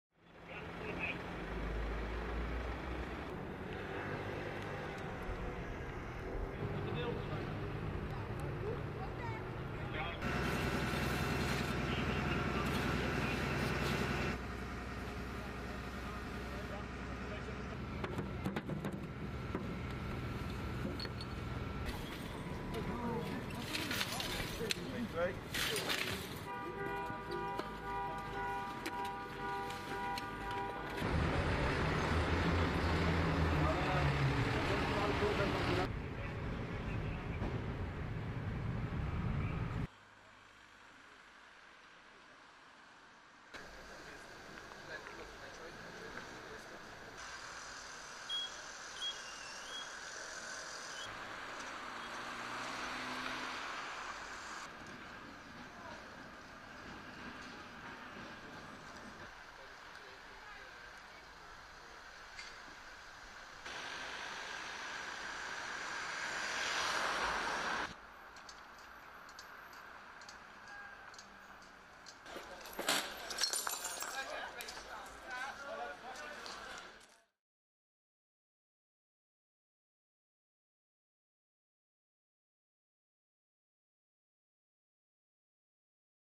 Trail of destruction | Anti-lockdown protest aftermath in Eindhoven and the Hague - News 360 Tv
Cities of Eindhoven and the Hague deal with the aftermath of anti-lockdown protests that hit the Netherlands